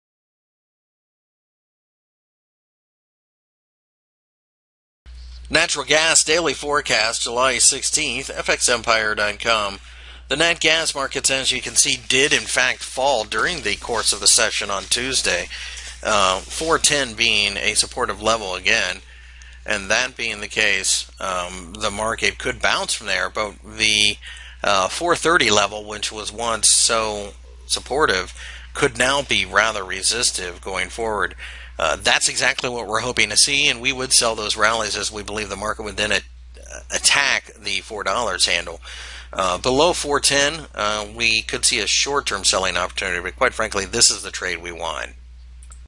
Natural gas Technical Analysis for July 16, 2014 by FXEmpire.com
- July 16, 2014 commodity daily technical analysis for the Natural gas commodity. Find more information about Forex and Commodities News Technical Analysis Fundamental Analysis and Brokers on FX Empire